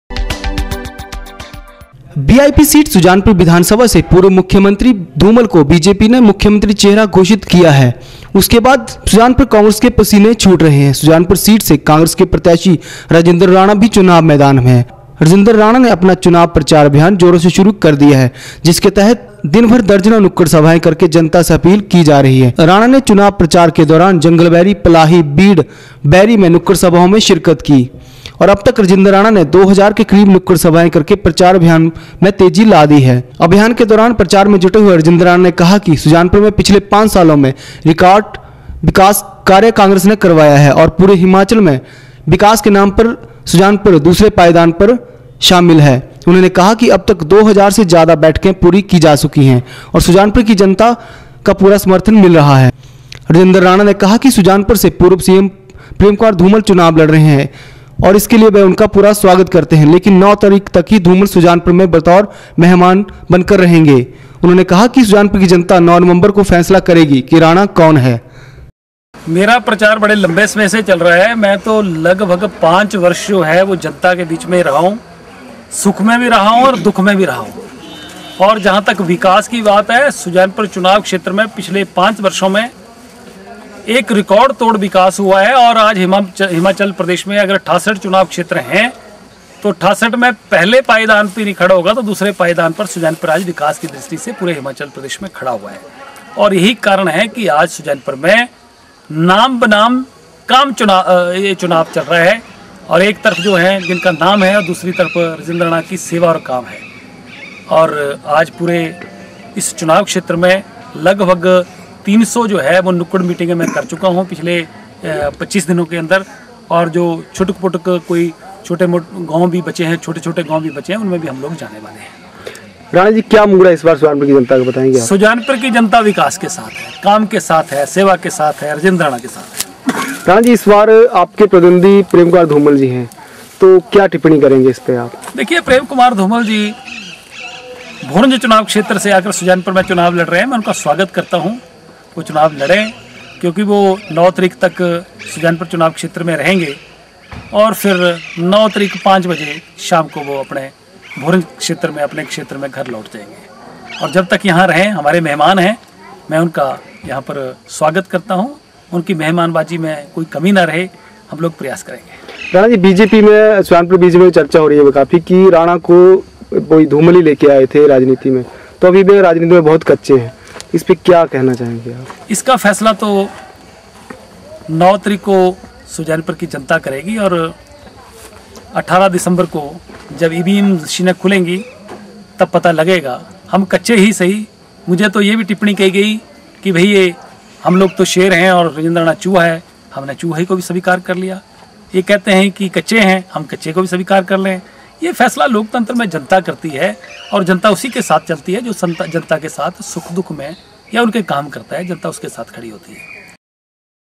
सुजानपुर सीट से कांग्रेस के प्रत्याशी राजेन्द्र राणा ने अपना चुनाव प्रचार अभियान में लाई तेजी ।
चुनावों की वीआइपी सीट सुजानपुर विधानसभा से पूर्व मुख्यमंत्री धूमल को बीजेपी ने मुख्यमंत्री चेहरा घोषित होने के बाद से  कांग्रेस के पसीने छूट रहे है। सुजानपुर सीट से कांग्रेस के प्रत्याशी राजेन्द्र राणा चुनाव मैदान में है।  राजेन्द्र  राणा ने अपना चुनाव प्रचार अभियान जोरों से शुरू कर दिया है जिसके तहत दिन  दर्जनो ंनुक्कड सभाएं  करके  जनता से अपील कर रहे है। राणा ने चुनाव प्रचार के दौरान जंगलबैरी, पलाही, बीड, में नुक्कड सभाओं में शिरकत की तो अब तक राजेन्द्र राणा ने 2000 के करीब नुक्कड सभाएं करके प्रचार अभियान में तेजी ला दी है।